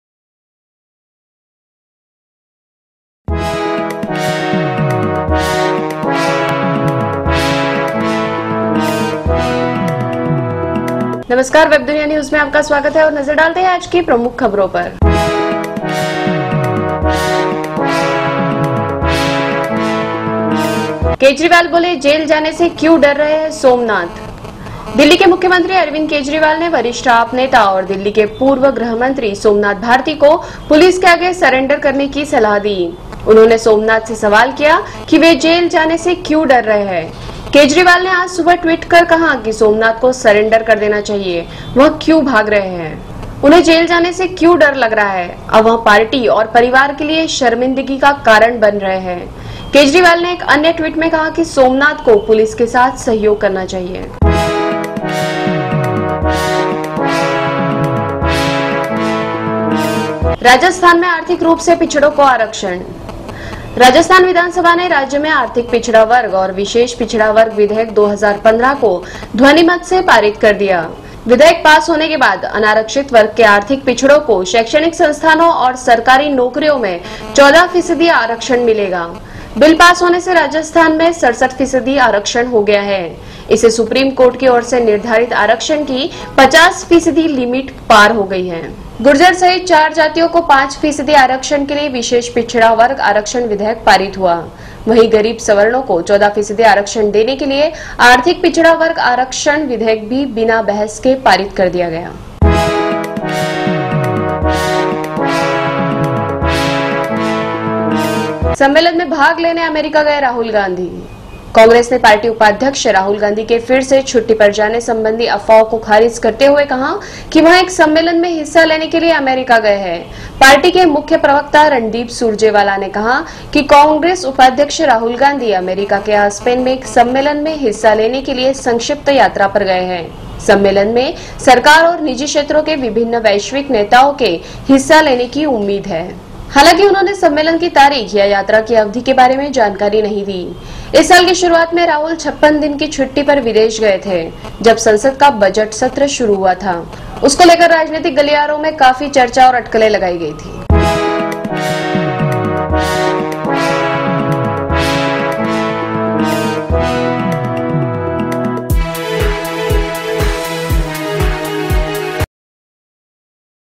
केजरीवाल बोले, जेल जाने से क्यों डर रहे हैं सोमनाथ | Somnath Bharti should surrender, Says Kejriwal
दिल्ली के मुख्यमंत्री अरविंद केजरीवाल ने वरिष्ठ आप नेता और दिल्ली के पूर्व गृहमंत्री सोमनाथ भारती को पुलिस के आगे सरेंडर करने की सलाह दी। उन्होंने सोमनाथ से सवाल किया कि वे जेल जाने से क्यों डर रहे हैं? केजरीवाल ने आज सुबह ट्वीट कर कहा कि सोमनाथ को सरेंडर कर देना चाहिए। वह क्यों भाग रहे हैं? उन्हें जेल जाने से क्यों डर लग रहा है? अब वह पार्टी और परिवार के लिए शर्मिंदगी का
कारण बन रहे हैं। केजरीवाल ने एक अन्य ट्वीट में कहा कि सोमनाथ को पुलिस के साथ सहयोग करना चाहिेए।